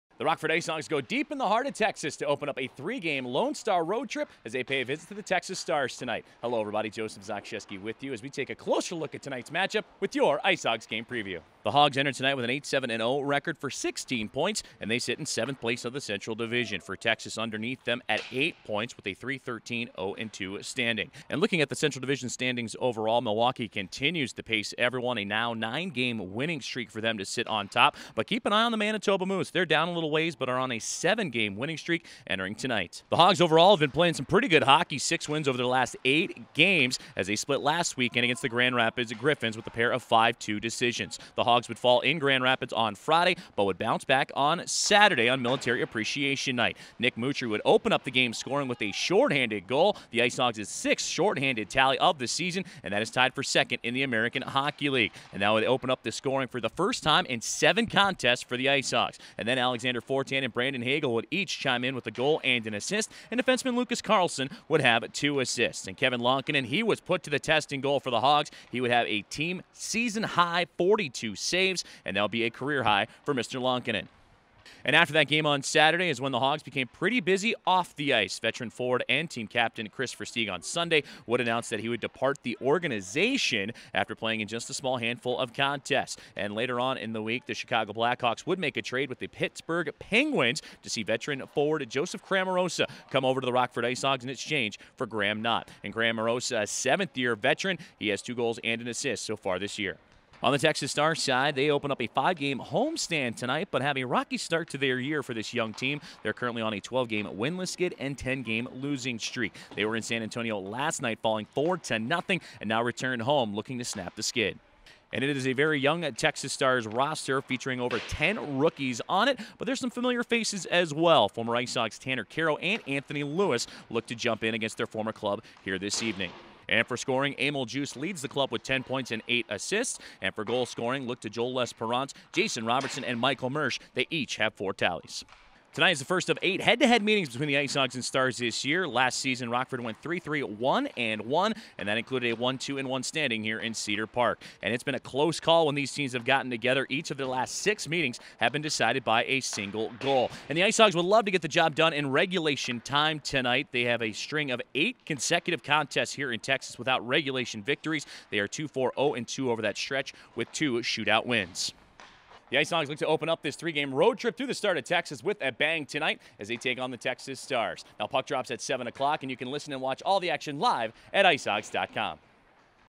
IceHogs Game Preview: 11/23/19 at Texas
Game Preview 🎥: IceHogs open three-game swing through Texas tonight as they visit the Texas Stars. Puck drops at 7!